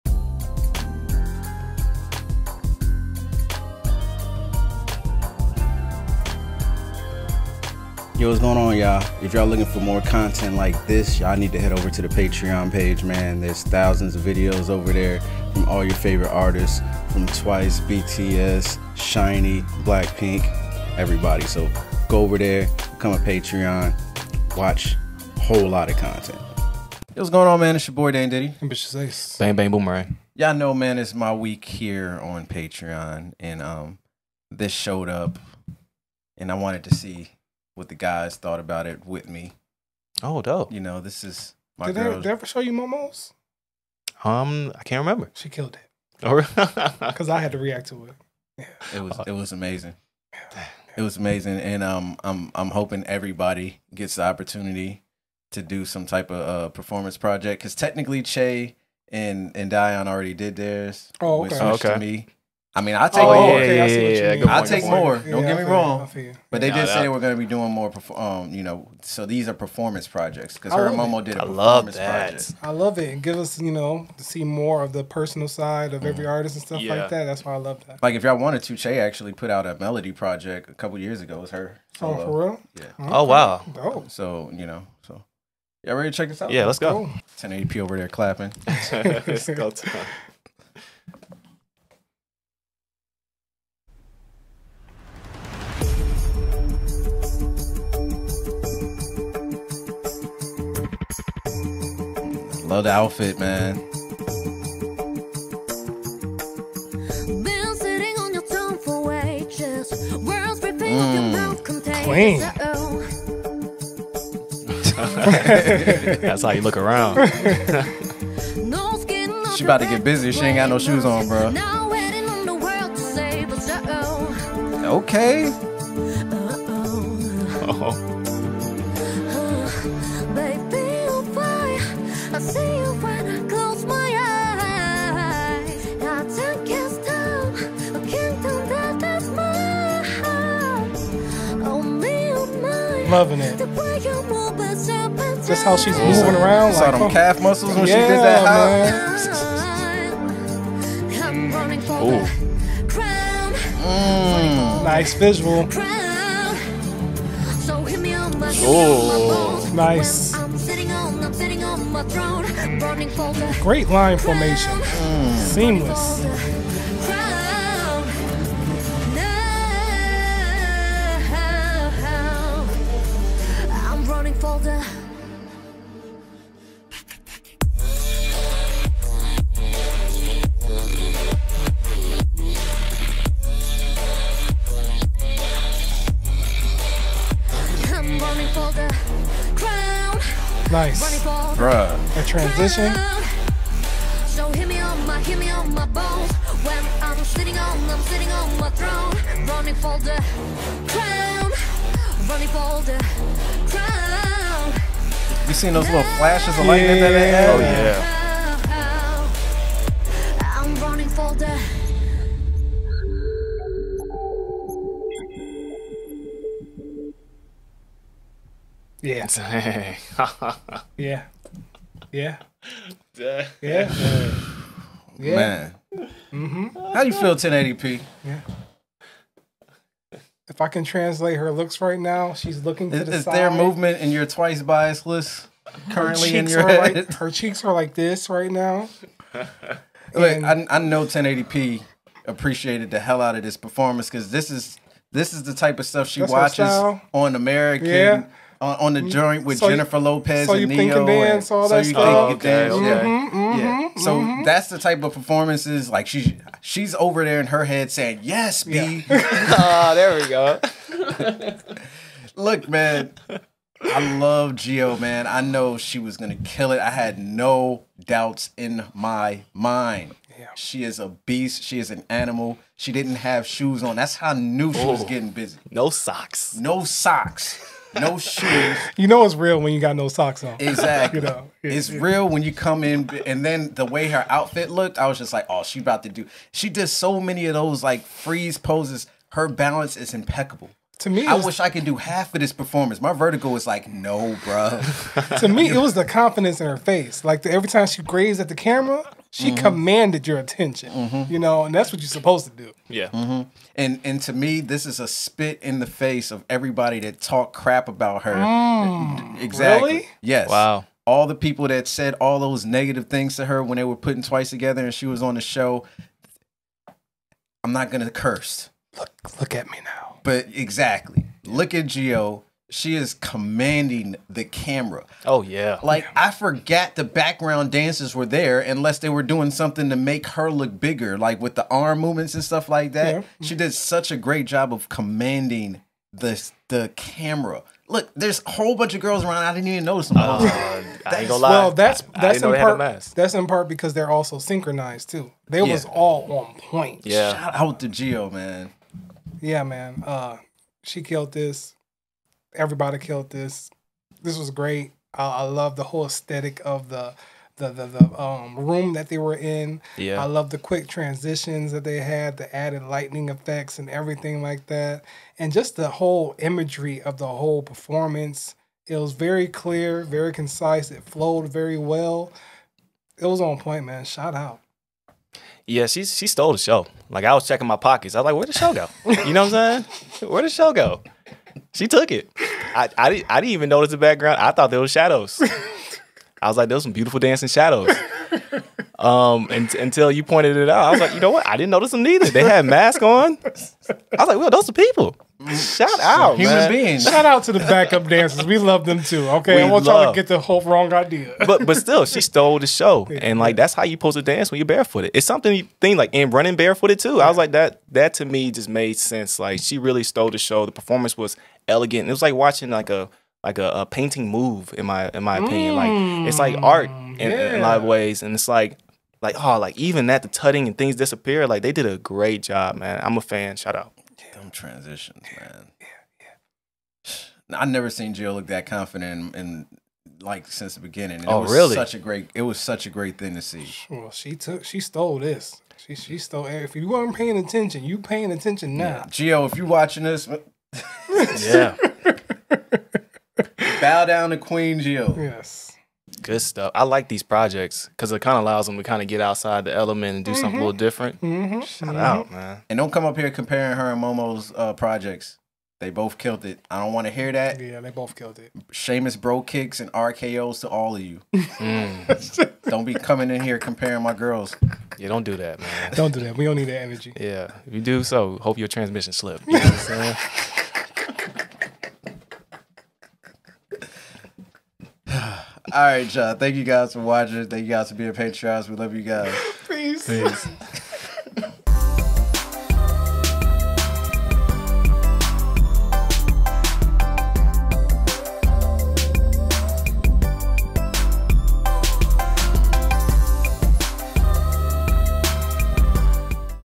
Jihyo Performance Project Reaction
We sat down to check out Jihyo doing her Performance Project! Twice is just amazing! Lets see how this goes!

Grade Scale

Check

Dash

X